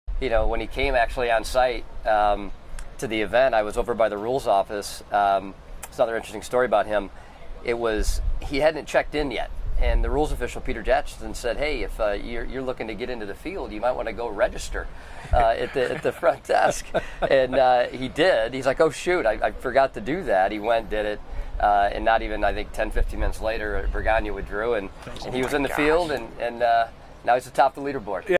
Nate Lashley almost missed his chance to play in the Rocket Mortgage Classi
Nate Lashley almost missed his chance to play in the Rocket Mortgage Classi. Jason Langwell shares more.